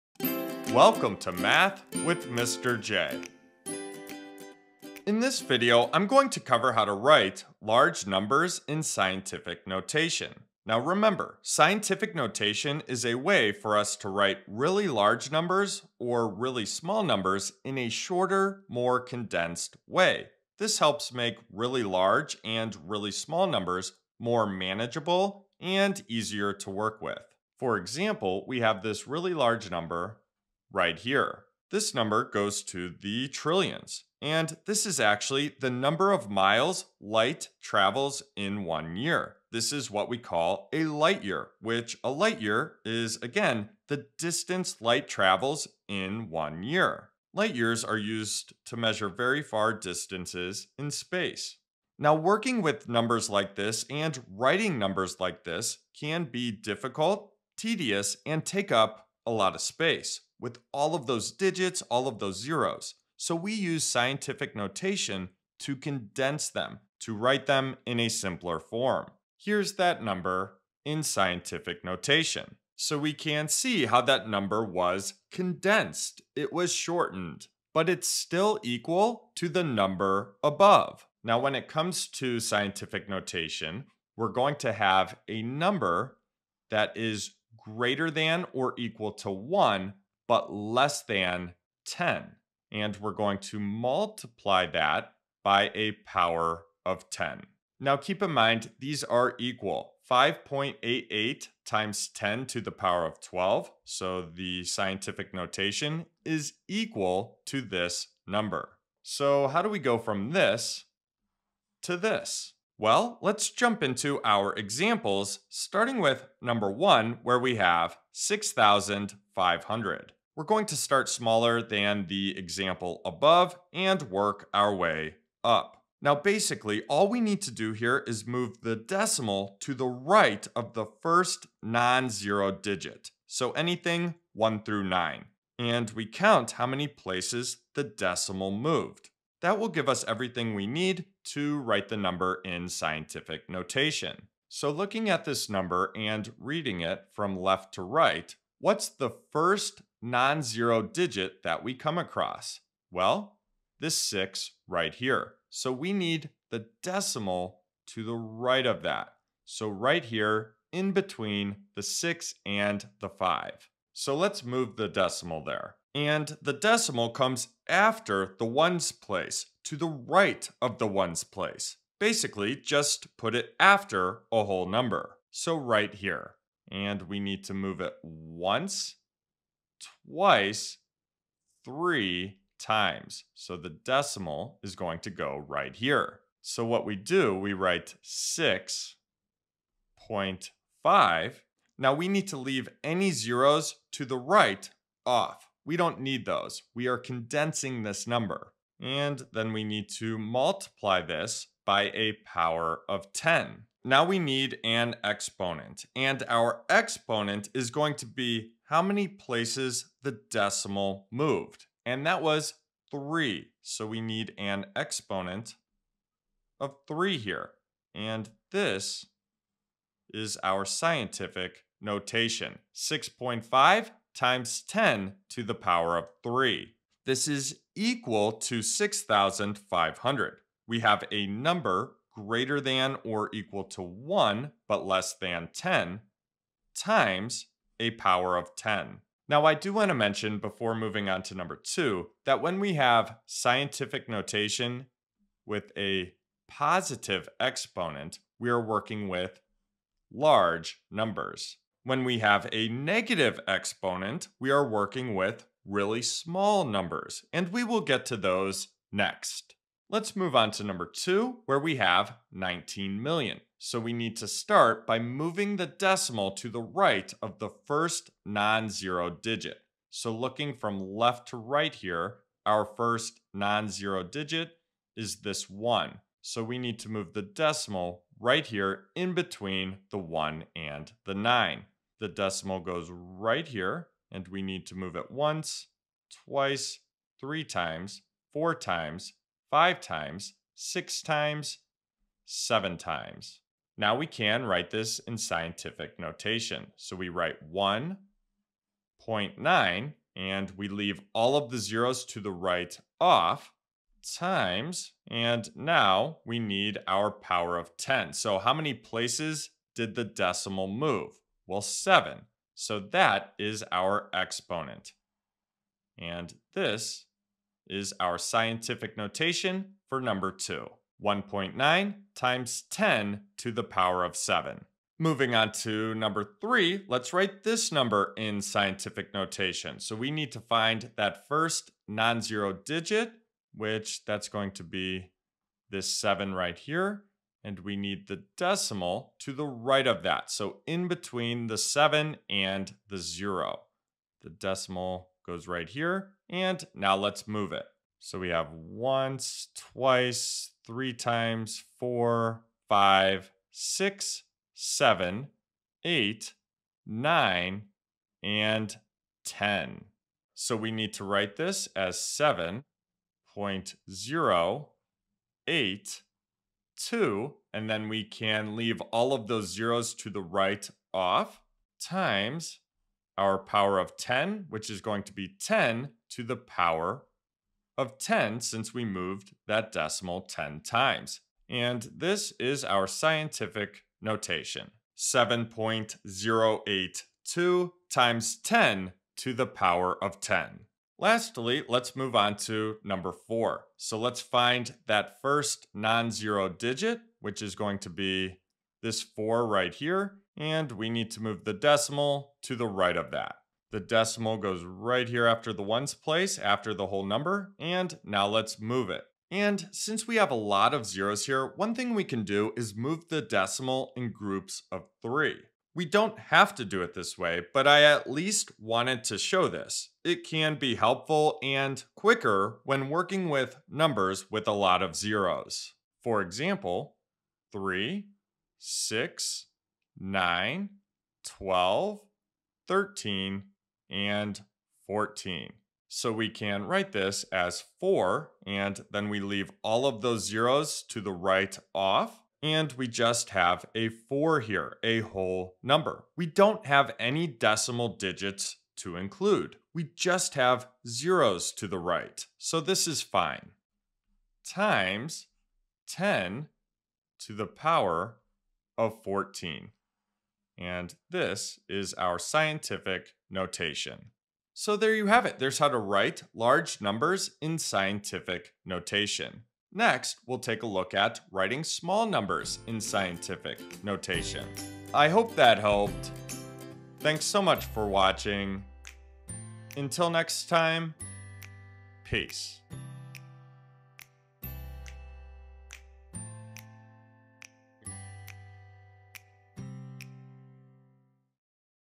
Scientific Notation | Writing Large Numbers in Scientific Notation | Math with Mr. J
Welcome to Writing Large Numbers in Scientific Notation with Mr. J! Need help with scientific notation? You're in the right place!

Whether you're just starting out, or need a quick refresher, this is the video for you if you're looking for help with how to write large numbers in scientific notation. Mr. J will go through four examples of writing large numbers in scientific notation and the process will be explained step-by-step.

Hopefully this video is what you're looking for when it comes to scientific notation.